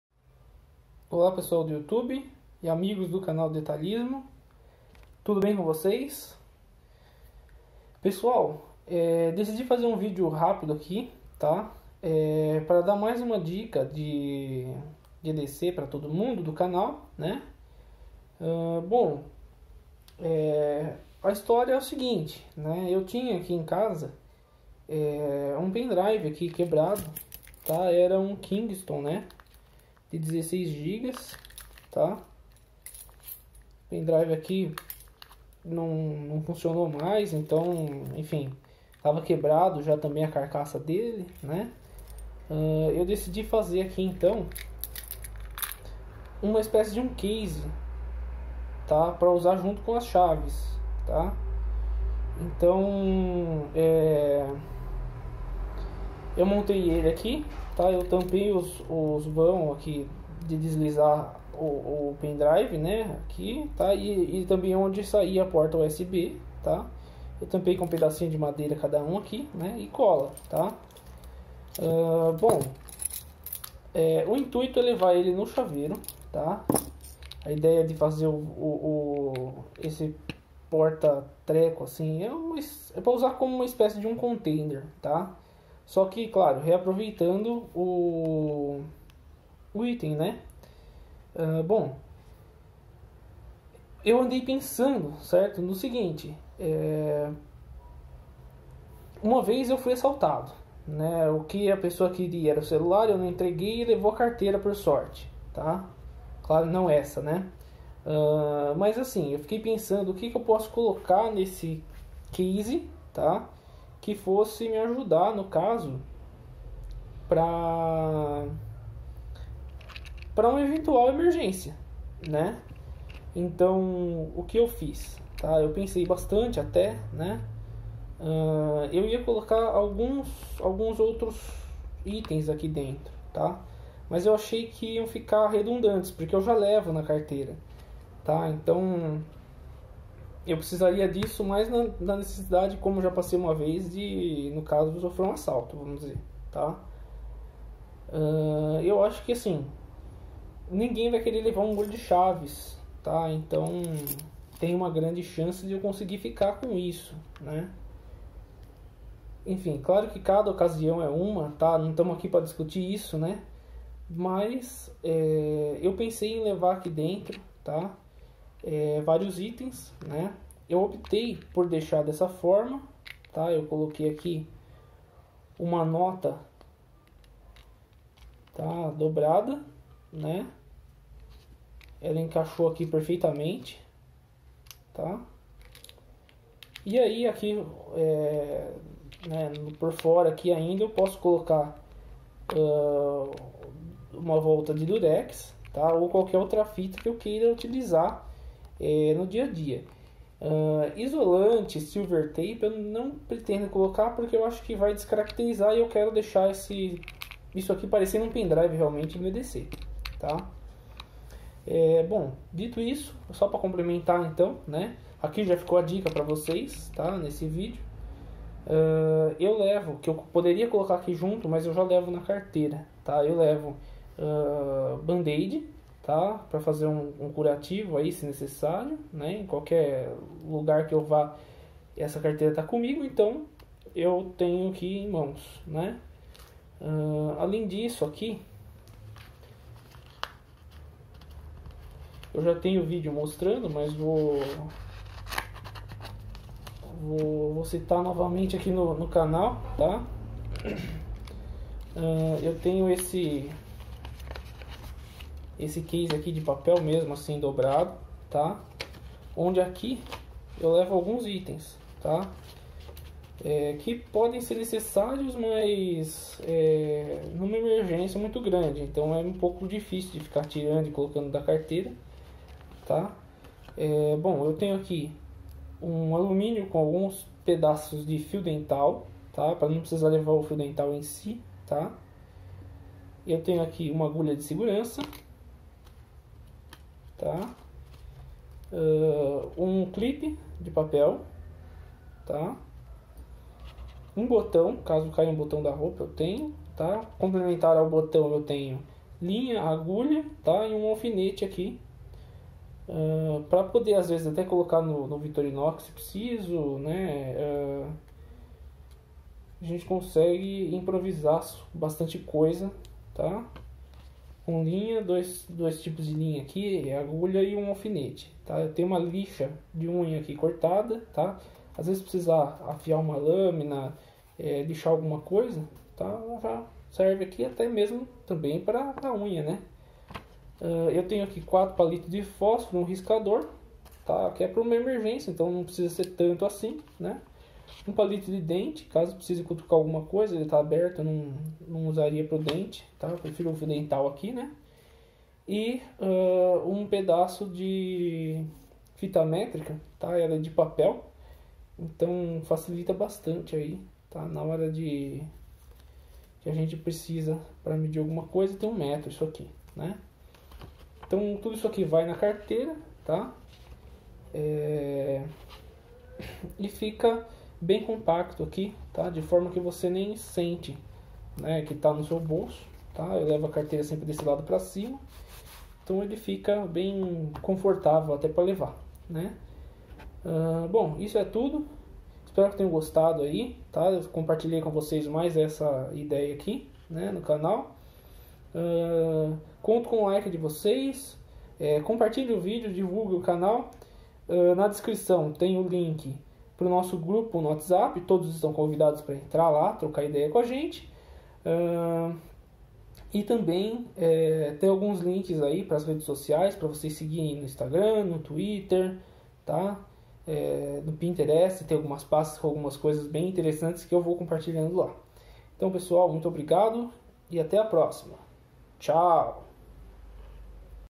Case de pendrive no chaveiro EDC e itens que levo na carteira - EDC Canal Detalhismo
Olá amigos do Canal Detalhismo!

Espero que todos estejam bem. Neste vídeo, mais ideias para seu EDC de carteira ou chaveiro.

***Acesse nosso grupo no WhatsApp, novo grupo de Victorinox e EDC do Canal Detalhismo

Grande abraço!!!

Marcos
Canal Detalhismo